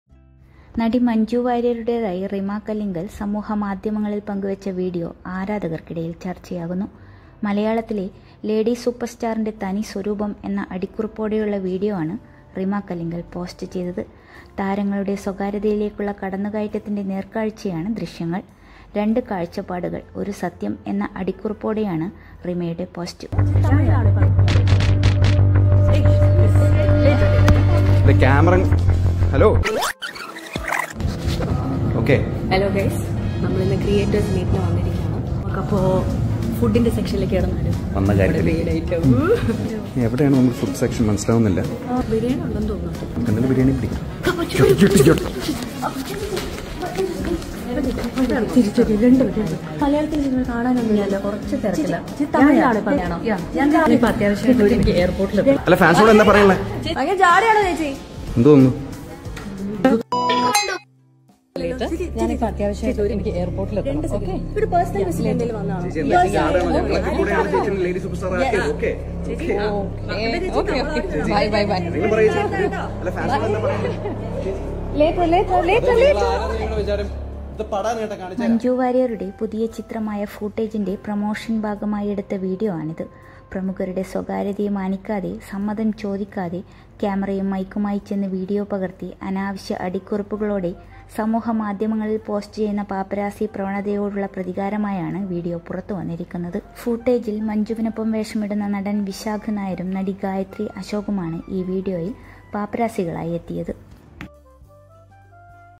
മഞ്ജുവര്യർ കലിപ്പിൽ യൂട്യൂബറോട് ചെയ്തത്.., വൈറലായി വീഡിയോ! /manjuwarier latest video
മലയാളത്തിലെ ലേഡി സൂപ്പർസ്റ്റാറിന്റെ തനി സ്വരൂപം’; വൈറലായി റിമ കല്ലിങ്കലിന്റെ വിഡിയോ...